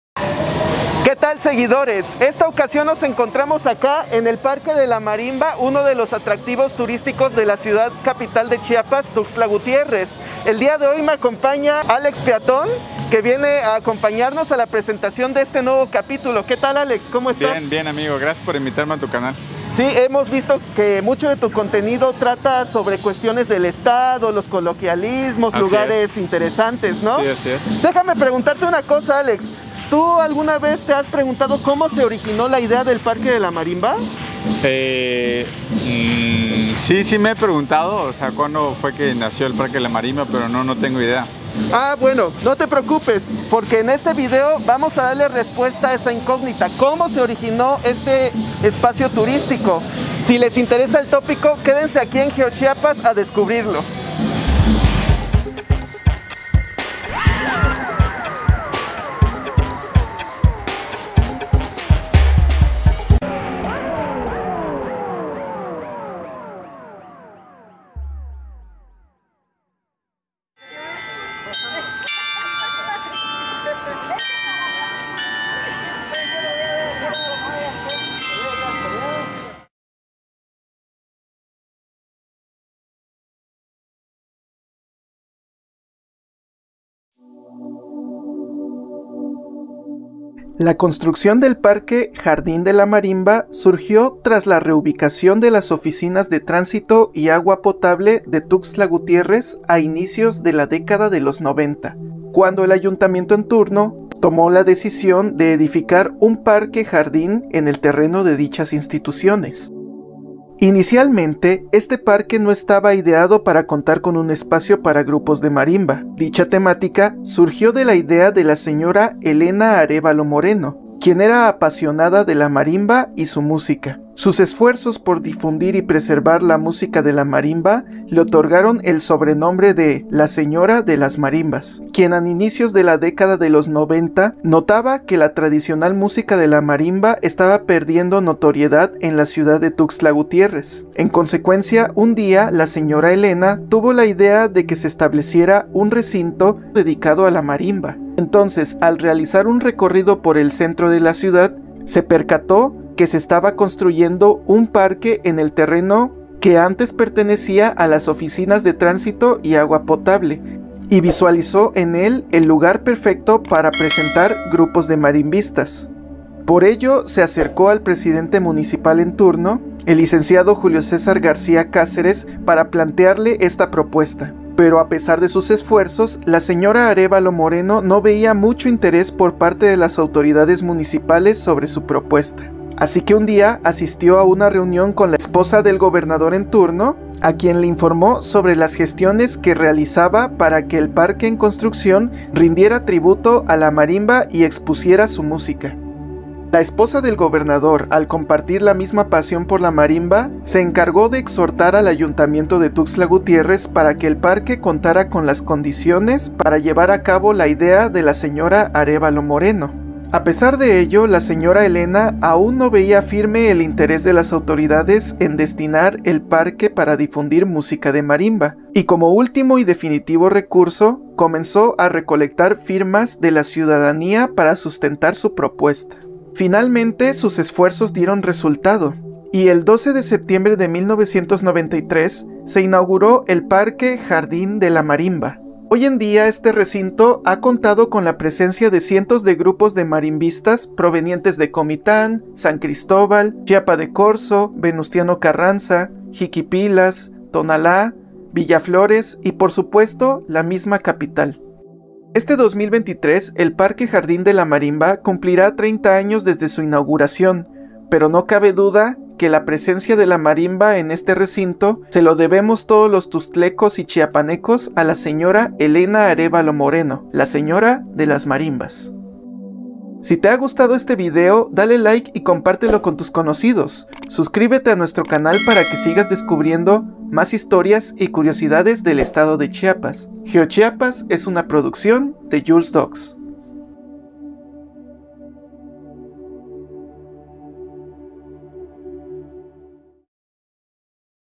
El Origen del Parque de la Marimba | GeoChiapas
Te has preguntado ¿Cómo se originó el parque jardín de la Marimba de Tuxtla Gutiérrez? descúbrelo en este video.

Un enorme agradecimiento a Alex Peatón por sus impresionantes tomas

ESCENAS DEL VIDEO:
Presentación inicial: 00:00
Intro: 00:54
El Origen del Parque de la Marimba: 01:21